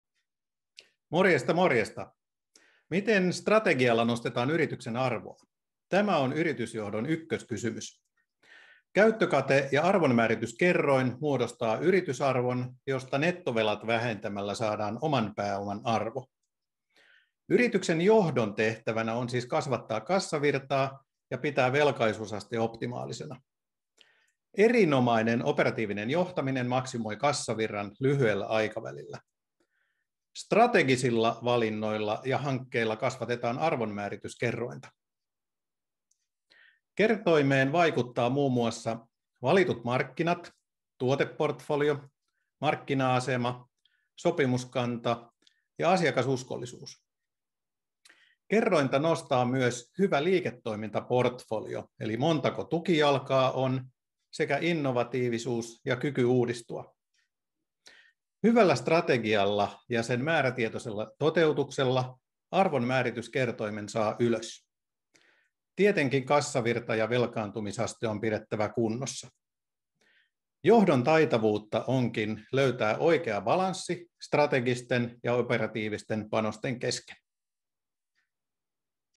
Miten strategialla vaikutetaan yritysarvoon
Miten strategialla nostetaan yrityksen arvoa? Tämä on yritysjohdon ykköskysymys. Käyttökate ja arvonmäärityskerroin muodostavat yritysarvon, josta nettovelat vähentämällä saadaan oman pääoman arvo. Yrityksen johdon tehtävänä on siis kasvattaa kassavirtaa ja pitää velkaisuusaste optimaalisena. Erinomainen operatiivinen johtaminen maksimoi kassavirran lyhyellä aikavälillä. Strategisilla valinnoilla ja hankkeilla kasvatetaan arvonmäärityskerrointa. Kertoimeen vaikuttavat mm. valitut markkinat, tuoteportfolio, markkina-asema, sopimuskanta ja asiakasuskollisuus.
Kerrointa nostavat myös hyvä liiketoimintaportfolio, eli montako tukijalkaa on, sekä innovatiivisuus ja kyky uudistua. Hyvällä strategialla ja sen määrätietoisella toteutuksella arvonmäärityskertoimen saa ylös.
Tietenkin kassavirta ja velkaantumisaste on pidettävä kunnossa.
Johdon taitavuutta onkin löytää oikea balanssi strategisten ja operatiivisten panosten kesken.